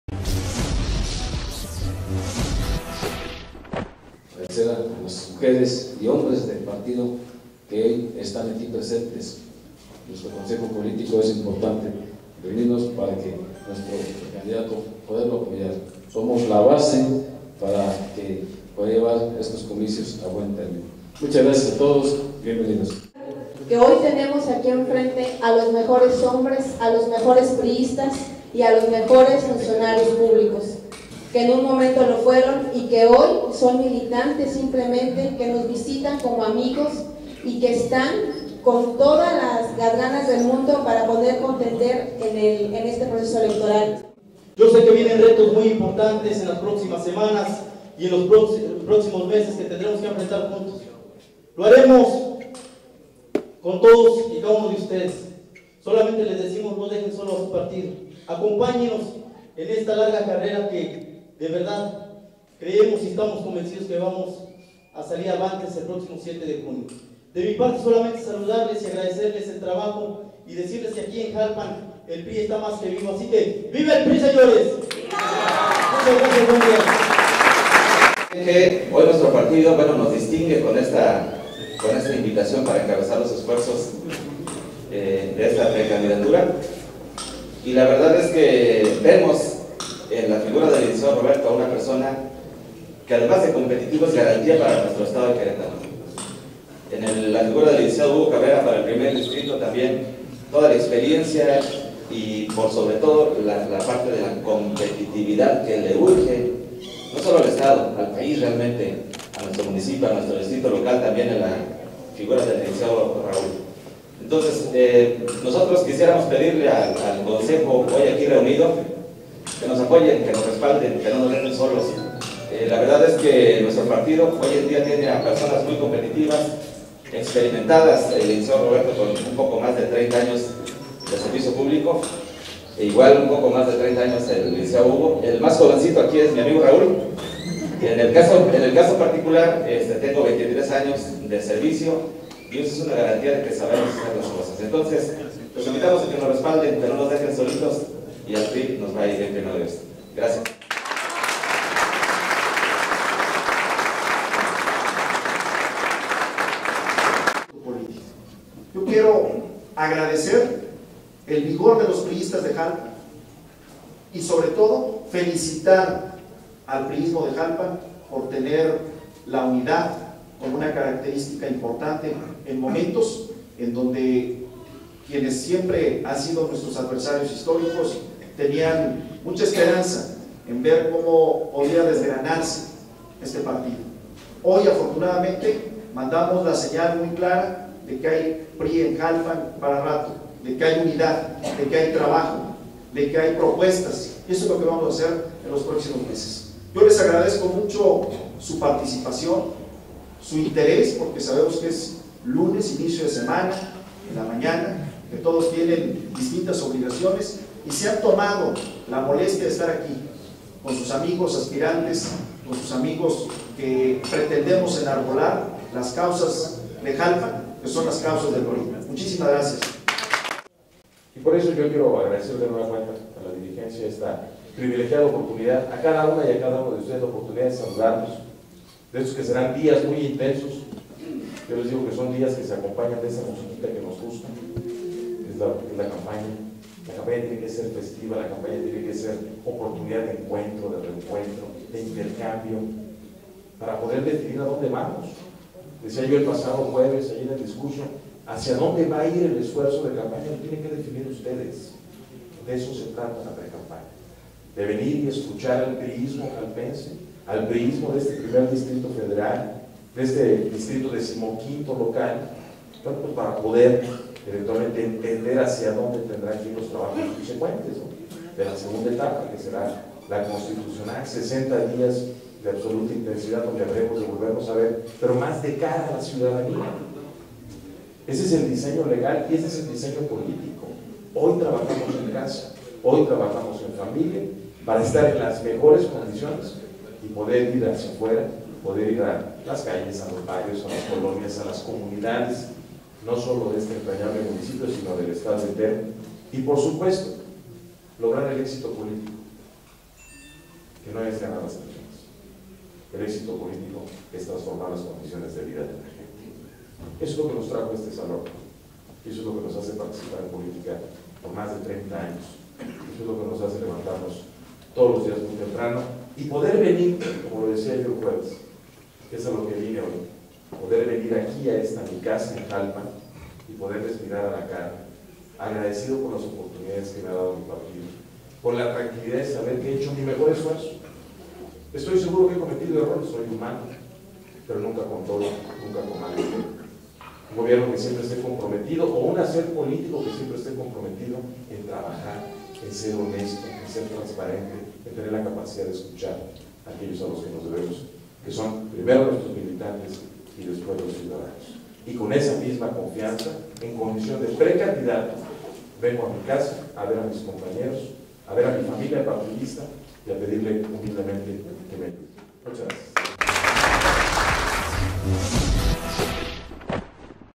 Reunión de Loyola con Consejo Político de Jalpan.
El Precandidato a la gubernatura del estado, Roberto Loyola Vera, sostuvo una reunión con Delegados del Consejo político del Partido Revolucionario Institucional, en donde recalcó la importancia que tiene, Jalpan y la Sierra Gorda para su proyecto político, mismo que compartirá con los precandidatos a la presidencia municipal de Jalpan, Manuel Ángeles Vega, Diputación Local por el XV Distrito, Raúl Uribe, Diputación Federal del 1er Distrito, Hugo Cabrera. En las instalaciones del PRI de Jalpan se llevó a cabo dicha reunión ante Delegados del Consejo Político del municipio. En su mensaje, Roberto Loyola Vera, convocó a todos los militantes de su partido para privilegiar a la unidad, comentó también que para cualquier acción es necesario recurrir al consejo, para que entre todos se brinden ideas, para realizar un mejor trabajo político.